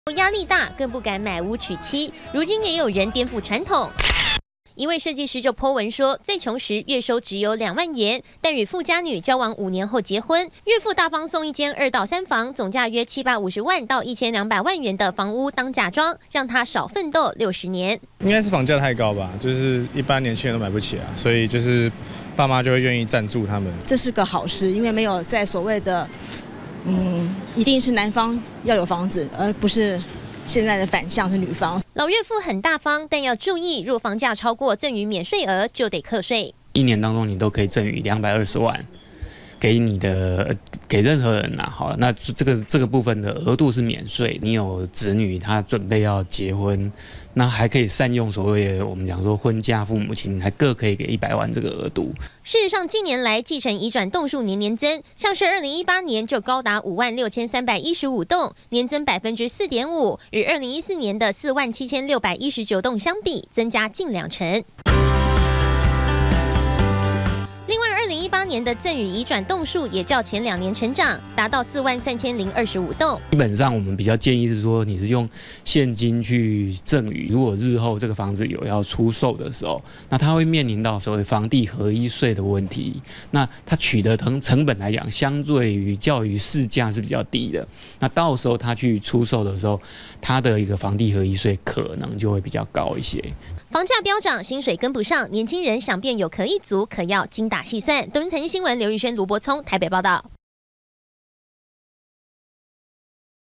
少奮鬥60年！ 娶富妻 岳父送房當嫁妝
房價飆漲讓小資族難以買房娶妻，不過現在有民眾靠娶有錢太太脫離貧困生活，結婚岳父還加碼送房，讓他大呼少奮鬥60年！